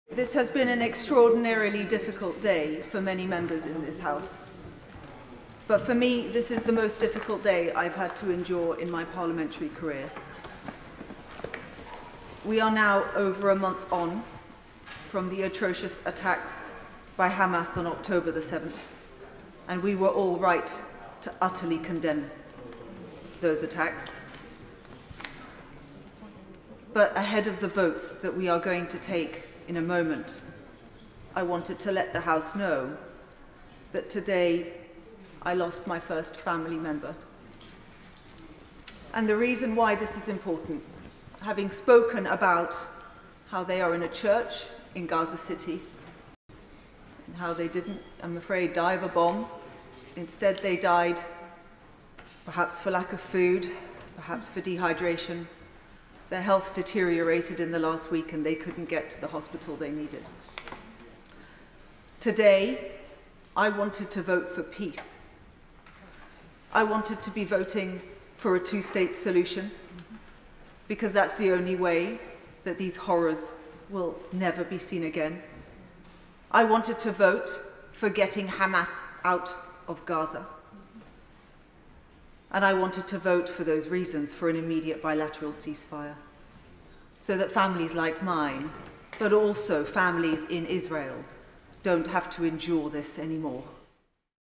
MP Layla Moran confirms death of family member in Gaza
Liberal Democrat MP Layla Moran has told the Commons one of her family members in Gaza has died.

The Oxford West and Abingdon MP, who claims to be the first MP of Palestinian descent, said it was the “most difficult day” she had had to endure in her parliamentary career.

She has previously shared her worries about her family members trapped in Gaza.

Ms Moran told the Commons: “I wanted to let the House know that today I lost my first family member.

“The reason why this is important, having spoken about how they are in a church in Gaza City and how they didn’t, I am afraid, die of a bomb, instead they died perhaps for lack of food, perhaps for dehydration."